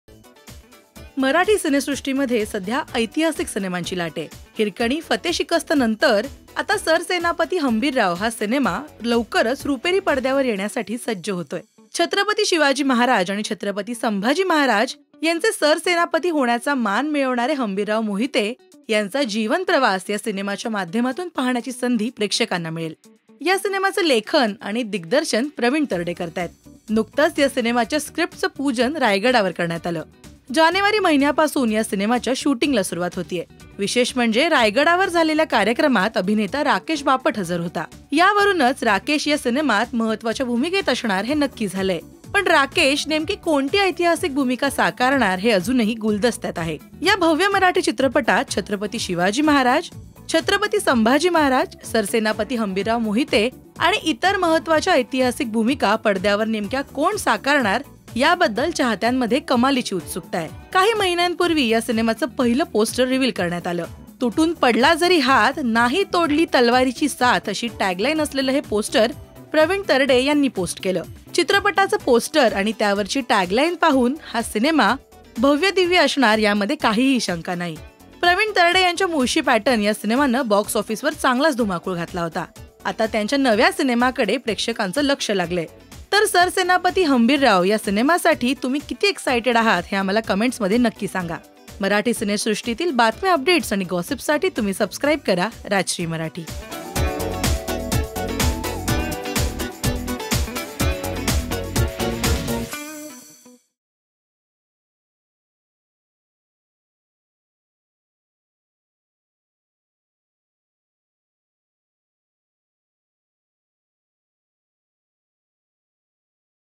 सरसेनापती हंबीरराव सिनेमात राकेश बापट | Raqesh Bapat In Pravin Tarde's SARSENAPATI HAMBIRRAO
सरसेनापती हंबीरराव' सिनेमाच्या स्क्रिप्टचं पूजन रायगडावर करण्यात आलं. या सिनेमात अभिनेता राकेश बापट महत्वाच्या भूमिकेत आहे. Watch this video to know more.

eporter- Darshana Tamboli,
Video Editor- Mahesh Mote